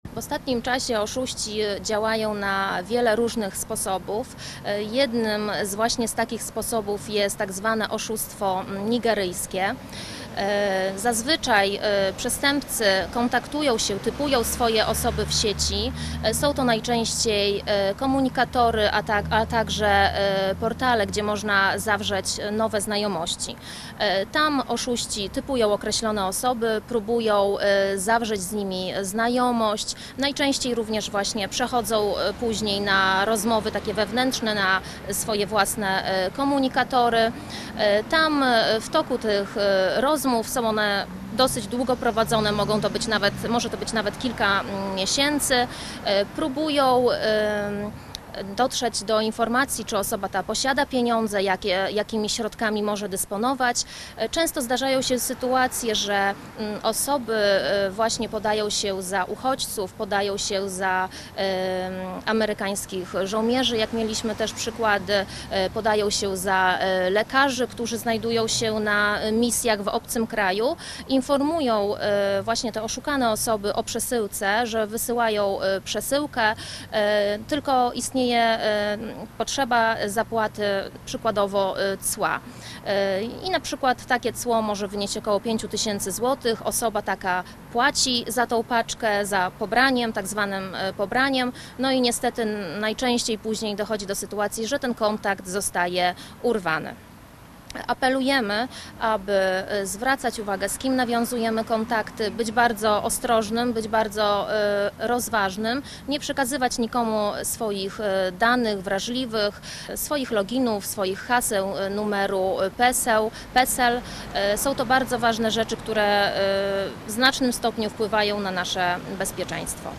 Nowa metoda oszustw internetowych | Telewizja Kanał S
Mimo wielu akcji profilaktycznych cały czas padamy ofiarą oszustów, często tracąc nawet oszczędności życia. Przestępcy w sieci są bardzo przebiegli i stosują coraz sprytniejsze metody oszustw, nawiązując z ofiarą nawet kilkumiesięczny kontakt. O wyłudzaniu pieniędzy metoda „oszustwa nigeryjskiego” w poniższym materiale.